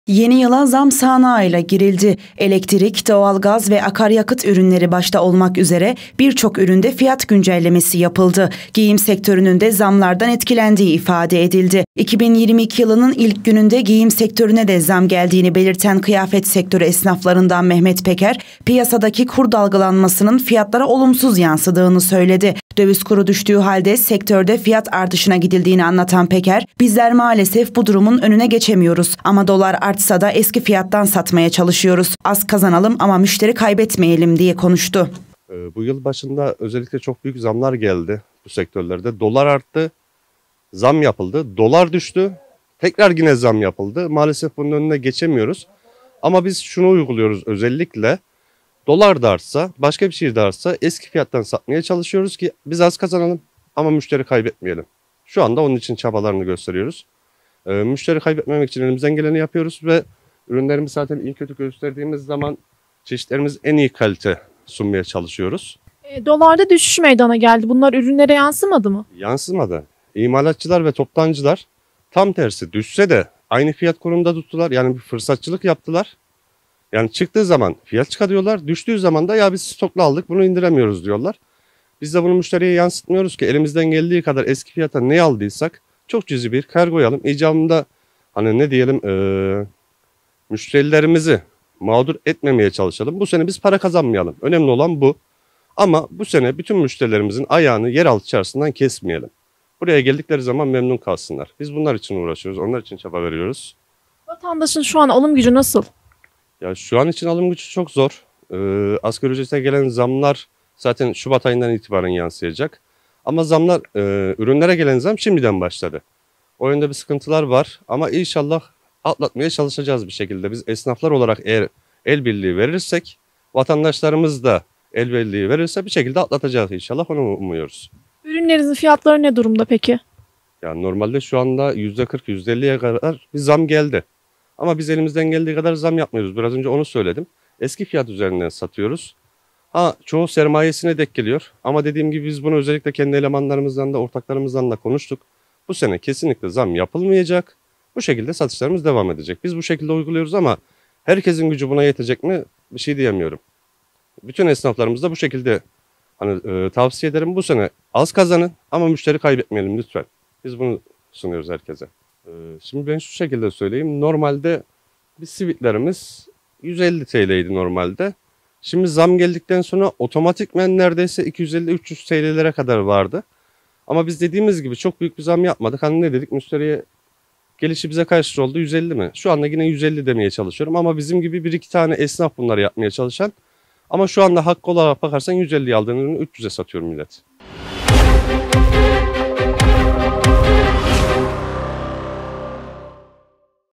Giyim Sektörüne Yüzde 50 Zam
KAYSERİ, (BGC- ÖZEL HABER) – Yeni yıla zam sağanağıyla girildi. Elektrik, doğal gaz ve akaryakıt ürünleri başta olmak üzere birçok üründe fiyat güncellemesi yapıldı. Giyim sektörünün de zamlardan etkilendiği ifade edildi.

2022 yılının ilk gününde giyim sektörüne de zam geldiğini belirten kıyafet sektörü esnaflarından Mehmet Peker, piyasadaki kur dalgalanmasının fiyatlara olumsuz yansıdığını söyledi. Döviz kuru düştüğü halde sektörde fiyat artışına gidildiğini anlatan Peker, “Bizler maalesef bu durumun önüne geçemiyoruz ama Dolar artsa da eski fiyattan satmaya çalışıyoruz. Az kazanalım ama müşteri kaybetmeyelim " diye konuştu.

"İMALATÇILAR VE TOPTANCILAR FIRSATÇILIK YAPIYOR"

Giyse imalatı yapan bazı kurumlar ile bazı toptancıların fırsatçılık yaptığını iddia eden Peker, dövizdeki düşüşe rağmen zamların geri çekilmediğini savundu. Peker, "İmalatçılar ve toptancılar bu süreçte doların düşmesine rağmen fırsatçılık yaptı. Bunlar dolar arttığı zaman fiyatları arttırdılar dolar düştüğü zaman da ‘stoklu aldık biz fiyatları indiremiyoruz’ diyorlar" ifadelerini kullandı.

"YENİ YILDA ÜRÜNLERE YÜZDE 50'YE YAKIN BİR ZAM GELDİ"

Artan fiyatlar nedeniyle vatandaşın alım gücünün iyice düştüğünü anlatan Peker, şu ana kadar kıyafet ürünlerine yüzde 50 civarında zam geldiğini açıkladı. Asgari ücret hatırlatması da yapan Peker, “Asgari ücrete gelen zamlar şubat ayından itibaren yansıyacak ama giysi ürünlerine gelen zamlar şimdiden başladı. El birliği yaparsak bu durumu atlatabiliriz" diye konuştu.